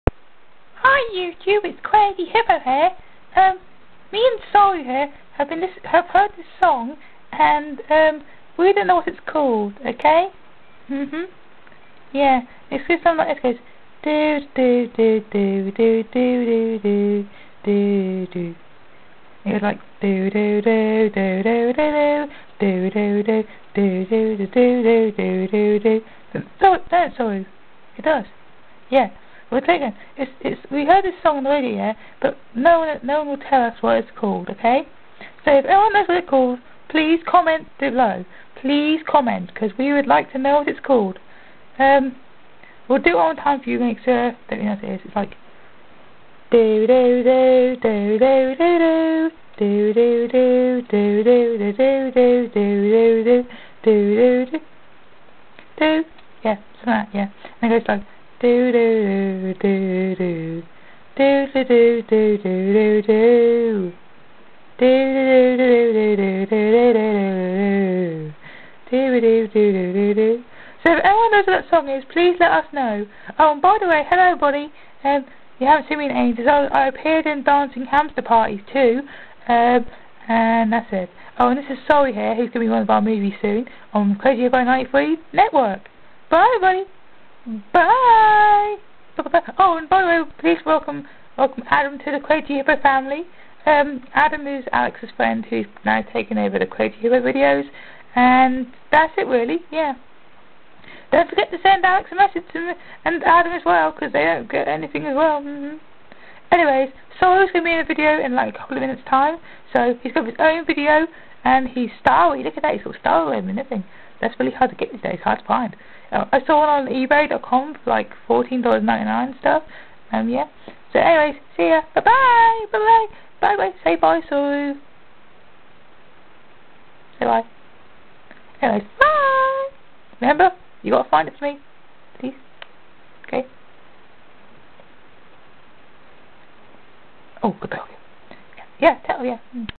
Crazy Hippo Needs Your Help!
Crazy Hippo wants to know what song he heard and he needs your help :p

Thanks

(Also welcoming Adam to the Crazy Hippo Family)